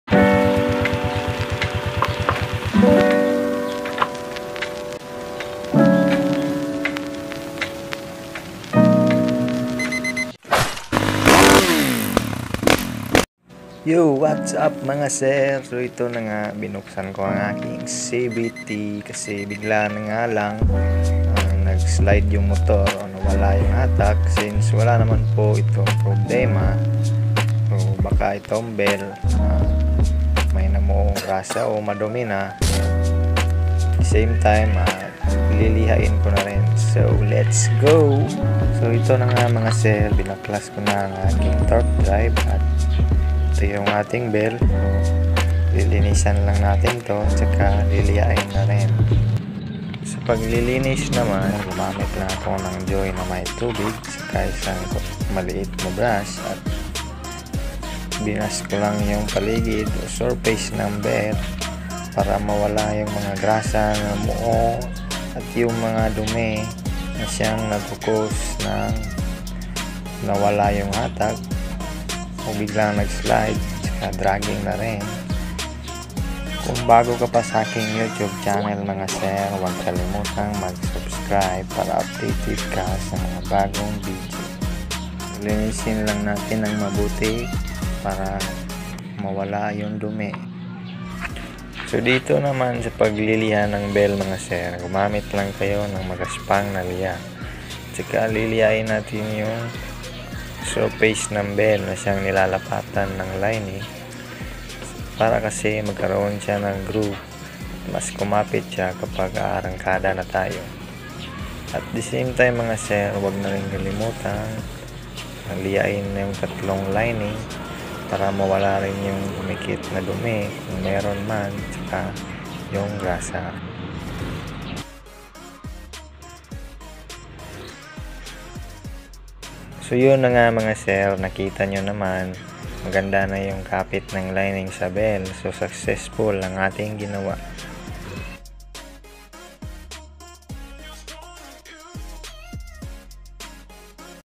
Dragging issue || maruming bell ng scooter || Rusi Gala Passion Royal
dragging Dragging problem ng scooter # Rusi Gala Rusi Passion # Rusi Royal

Dragging Issue sa scooter ay isa sa problema na naeecounter ito ay may maraming dahilan at sa video na ito ay ang bell na marumi na siyang dahilan ng pag dragging nito kaya ang mabisang pag ayus nito ay hugasan lamang ang surface ng bell para mawala ang namuong dumi o grasa na dahil nang ng pag slide ng lining kaya nagkak dragging